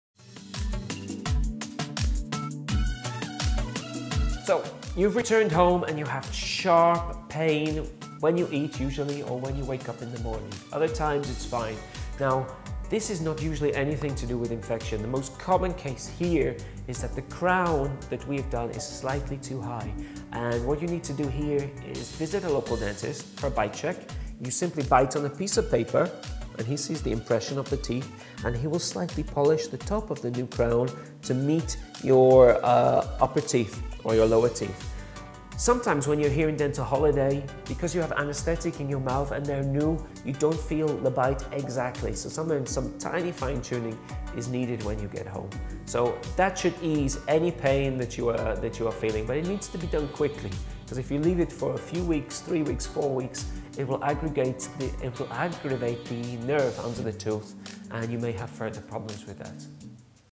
experiencing a sharp pain after having a dental crown done
If you returned home and you have sharp pain when you eat or when you wake up in the morning and other times it's fine,  this is not usually anything to do with infection.
The most common case here is that the crown that we've done is slightly too high and what you need to do here is visit a local dentist or you can check it by simply bite on a piece of paper to see the impression of the teeth.  If you feel this kind of pain, it needs to be done quickly because if you leave it for a few weeks it can aggregate the nerve under the tooth and you may have further problems with that.